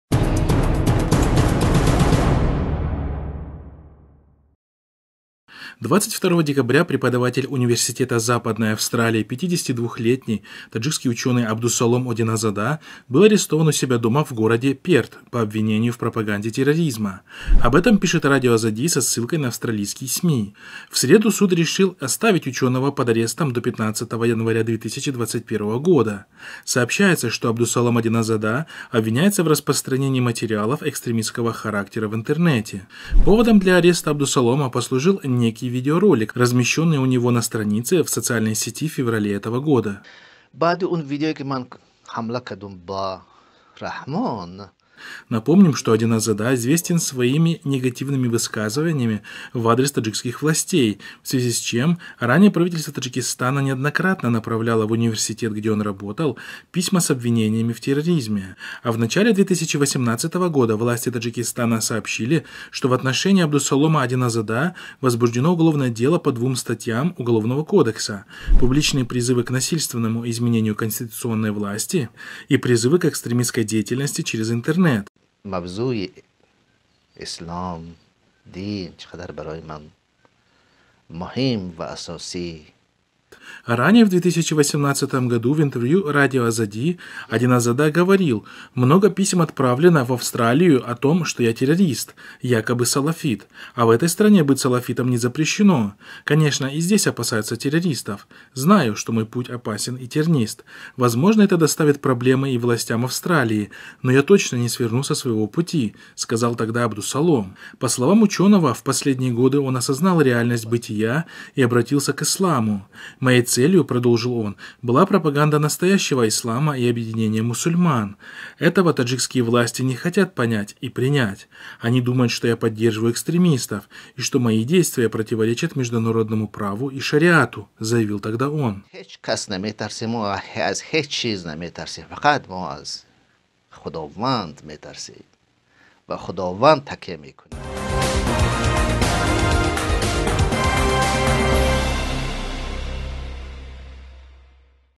В Австралии задержан ученый из Таджикистана
22 декабря преподаватель Университета Западной Австралии 52-летний таджикский ученый Абдусалом Одиназода был арестован у себя дома в городе Перт по обвинению в пропаганде терроризма, об этом пишет Радио Озоди со ссылкой на Австралийский СМИ.